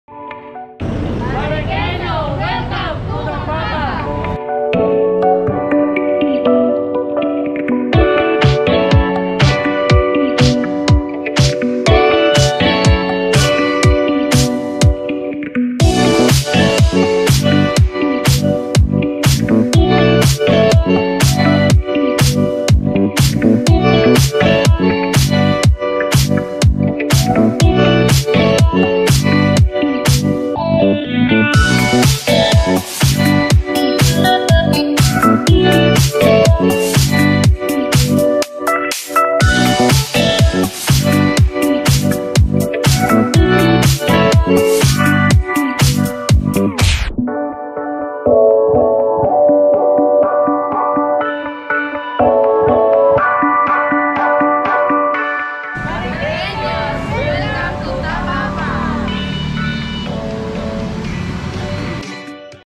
Tapapa Marikina
Tapapa is you go-to place if you are craving for anything. They have tapa, pares, fish, soup, taco, everything. You name it, they have it. Open from sunrise to sundown, from 6:30AM to 12:30AM, Mondays to Saturdays. Visit their store located along J.P. Rizal Street, Malanday (Lamuan) near Graceland Plaza. You can contact them via their Facebook and Instagram accounts or order via Food Panda and Grab Food.